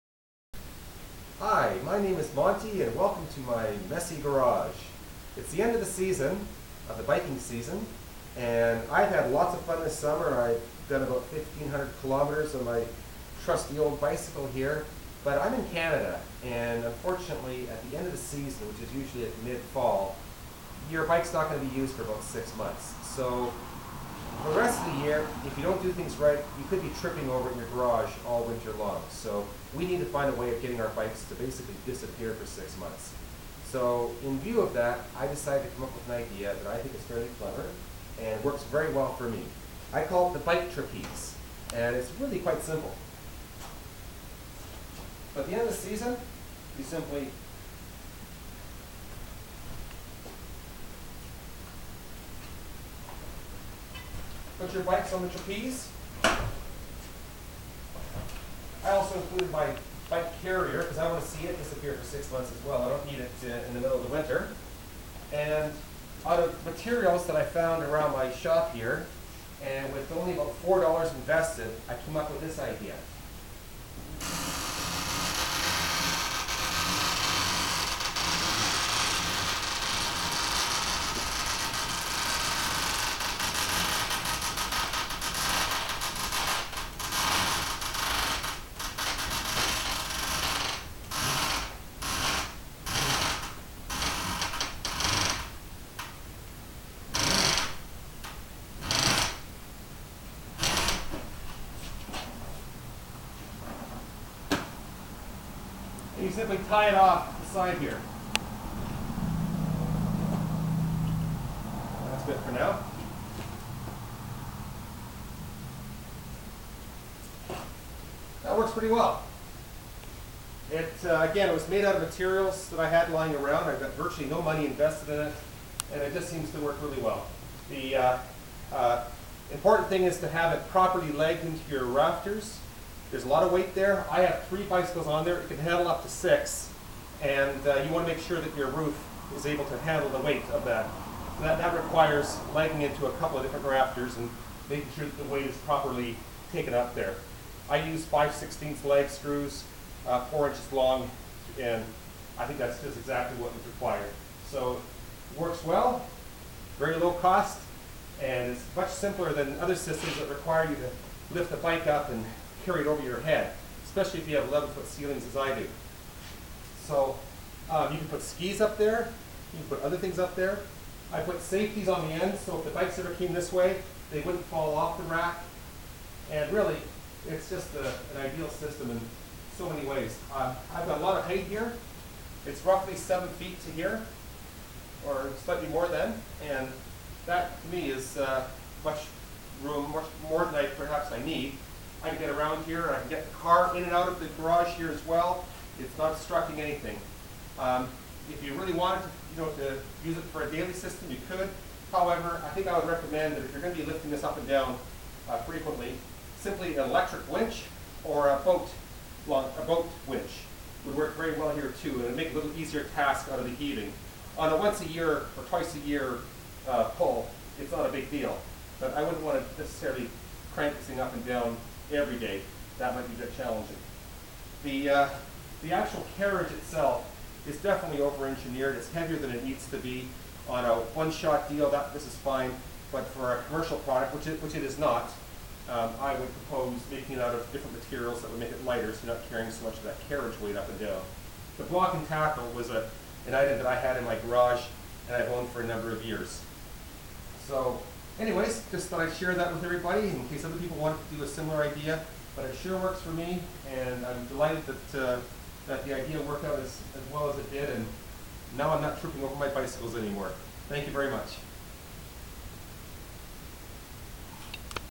"Bike Trapeze" Bicycle Storage System for Garage
This is a homemade bicycle storage system I devised from hardware and materials lying around my garage.  It utilizes a simple trapeze system coupled with a block-and-tackle to lift the bicycles that normally clutter up my shop during our Canadian winters and bring them up 7 feet in the air and out of reach.  It works remakably well as it is, although it is a "first try" and could easily be improved upon, perhaps by employing an electric or hand-cranked boat winch to cut down on muscle power..